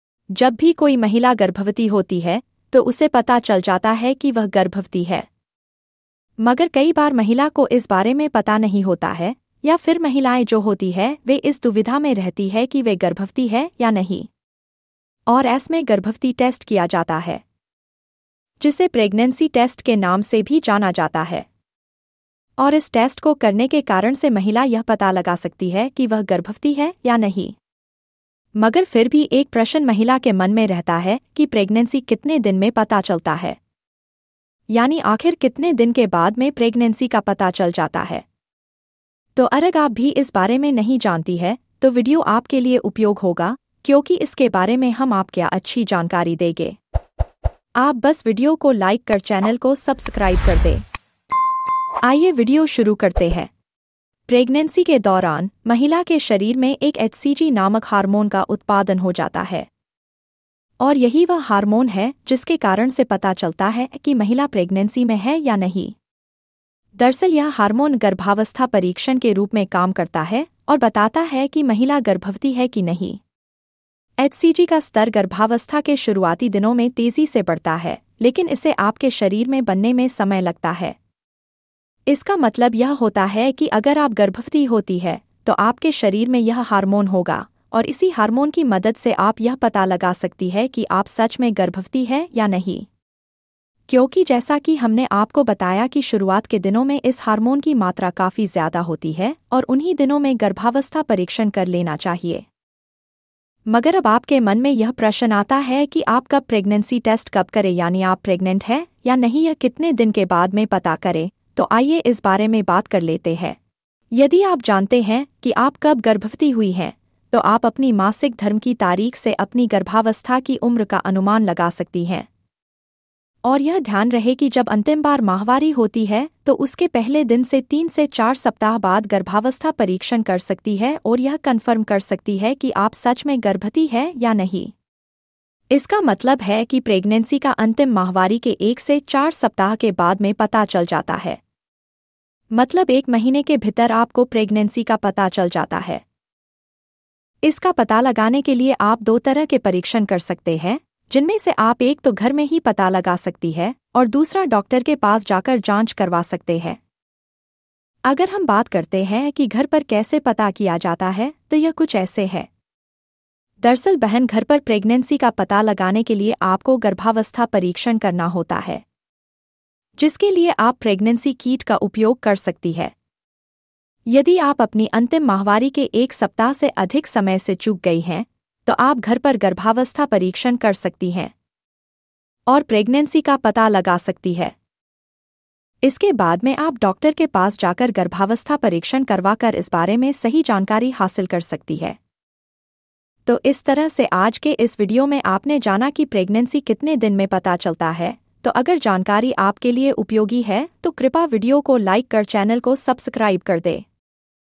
प्रेगनेंसी कितने दिन में पता चलता है | pregnancy kitne din mein pata chalta Hai
pregnancy kitne din me pata chalta hai
प्रेगनेंसी कितने दिन में पता चलता है

प्रेगनेंसी के दौरान, महिला के शरीर में एक एचसीजी नामक हार्मोन का उत्पादन हो जाता है । और यही वह हार्मोन है जिसके कारण से पता चलता है की महिला प्रेगनेंसी में है या नही । दरसल यह हार्मोंन गर्भावस्था परीक्षण के रूप में काम करता है और बताता है की महिला गर्भवती है की नही ।

 एचसीजी का स्तर गर्भावस्था के शुरुआती दिनों में तेजी से बढ़ता है, लेकिन इसे आपके शरीर में बनने में समय लगता है। इसका मतलब यह होता है की अगर आप गर्भवती होती है, तो आपके शरीर में यह हार्मोन होगा, और इसी हार्मोन की मदद से आप यह पता लगा सकती है की आप सच में गर्भवती है या नही । क्योकी जैसा की हमने आपको बताया की शुरुआत के दिनो में इस हार्मोन की मात्रा काफी ज्यादा होती है और उन्ही दिनो में गर्भावस्था परिक्षण कर लेना चाहिए ।

मगर अब आपके मन में यह प्रशन आता है की आप कब प्रेगनेंसी टेस्ट कब करे यानि आप प्रेगनेंट है या नही यह कितने दिन के बाद में पता करे, तो आइए इस बारे मे बात कर लेते है

यदि आप जानते हैं कि आप कब गर्भवती हुई हैं, तो आप अपनी मासिक धर्म की तारीख से अपनी गर्भावस्था की उम्र का अनुमान लगा सकती हैं। और यह ध्यान रहे की जब अंतिम बार माहवारी होती है तो उसके पहले दिन से 3 से 4 सप्ताह बाद गर्भावस्था परीक्षण कर सकती है ओर यह कन्फर्म कर सकती है की आप सच में गर्भती है या नही ।इसका मतलब है की प्रेगनेंसी का अंतिम माहवारी के एक से 4 सप्ताह के बाद में पता चल जाता है। मतलब एक महिने के भितर आपको प्रेगनेंसी का पता चल जाता है ।

इसका पता लगाने के लिए आप दो तरह के परिक्षण कर सकते है , जिनमें से आप एक तो घर में ही पता लगा सकती है, और दूसरा डॉक्टर के पास जाकर जांच करवा सकते है ।
अगर हम बात करते है की घर पर कैसे पता किया जाता है तो यह कुछ ऐसे है । दरसल बहन घर पर प्रेगनेंसी का पता लगाने के लिए आपको गर्भावस्था परीक्षण करना होता है ।

 जिसके लिए आप प्रेगनेंसी कीट का उपयोग कर सकती है ।  यदि आप अपनी अंतिम माहवारी के एक सप्ताह से अधिक समय से चूक गई हैं, तो आप घर पर गर्भावस्था परीक्षण कर सकती हैं। और प्रेगनेंसी का पता लगा सकती है ।